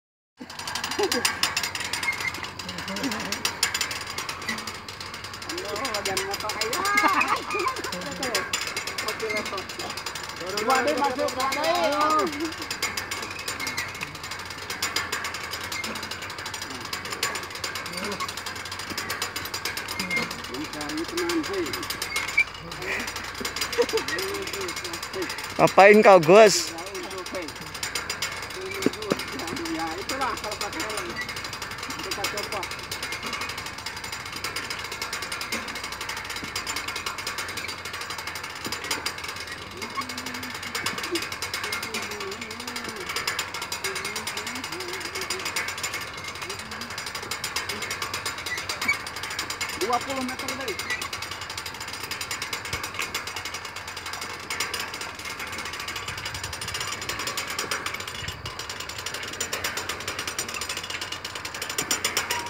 Erection tower
Proses erection tower walini bandung barat